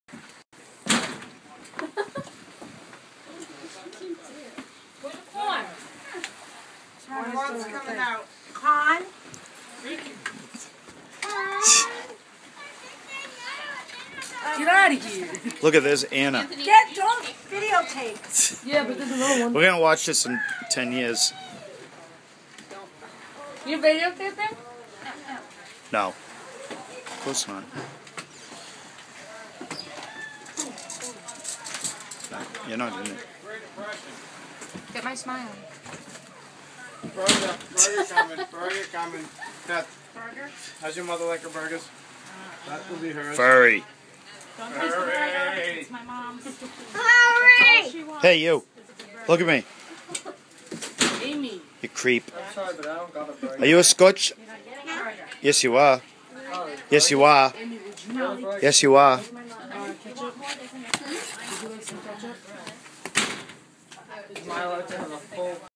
Lunch line at the lake
Lunch time at the lake. Angela's smile, Jerry cooking, and Anthony arguing about being a skootch!